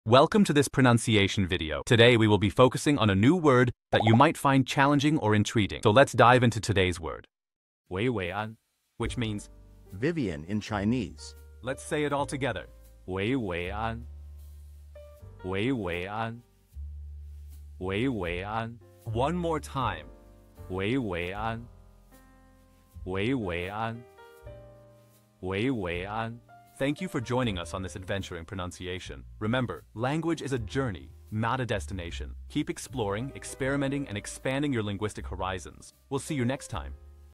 How to pronounce wéi wéi ān | 维维安 (Vivian in Chinese)
Master the Pronunciation of 'wéi wéi ān | 维维安' - which means : Vivian in Chinese 📝 with @PerfectPronunciationChinese 🎓🗣️ - [Your Guide to Chinese]

Learn to pronounce 'wéi wéi ān' perfectly in Chinese with our easy and engaging tutorial by @PerfectPronunciationChinese! 📖📽️ In this comprehensive video, we provide you with native speaker examples 🗣️👂 and phonetic breakdowns 🎵 to ensure you get the pronunciation just right.